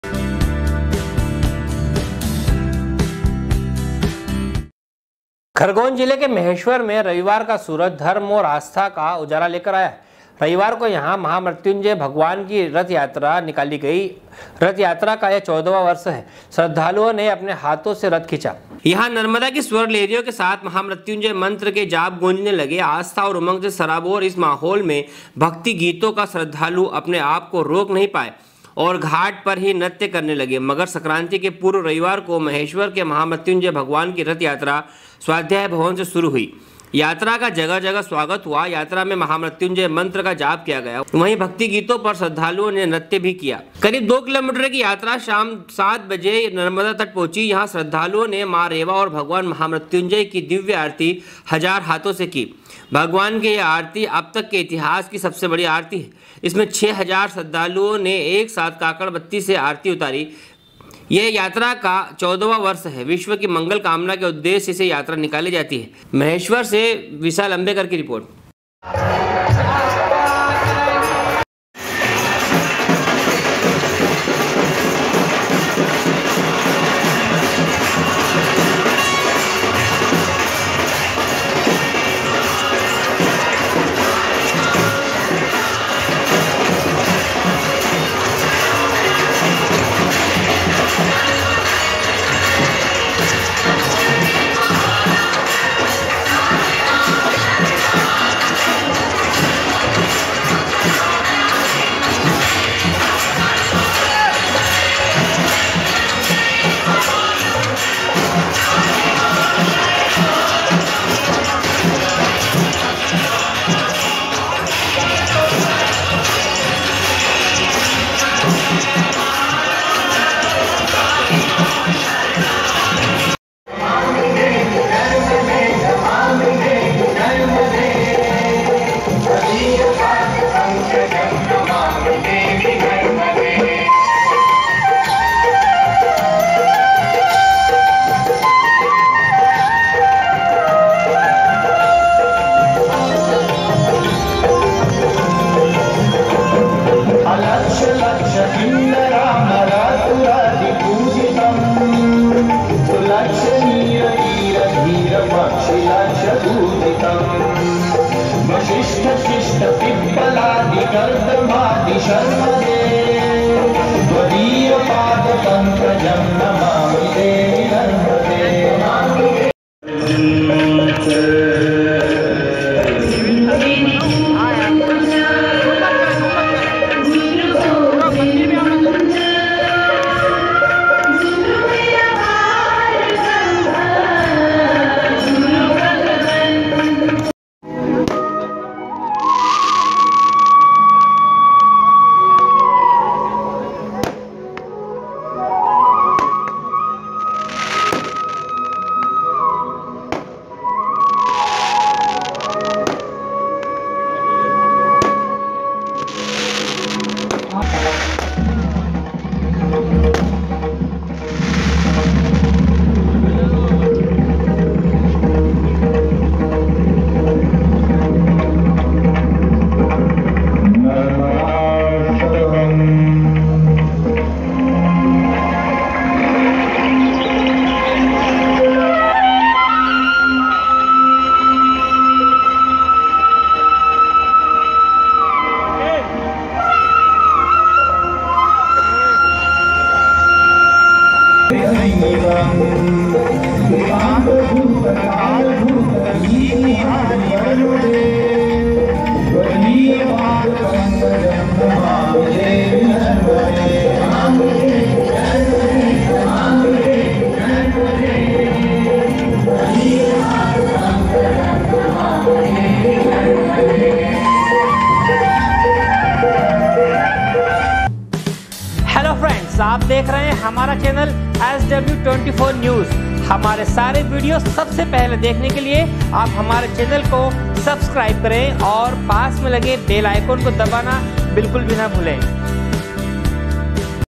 sw24news maheshwar, 18.01.2020
खरगोन जिले के महेश्वर में रविवार का सूरज धर्म और आस्था का उजाला लेकर आया रविवार को यहां महामृत्युंजय भगवान की रथ यात्रा निकाली गई यह यात्रा का 14वा वर्ष है श्रद्धालुओं ने अपने हाथों से रथ खींचा महेष्वर से विषाल अम्बेकर की रिपोर्ट#sw24news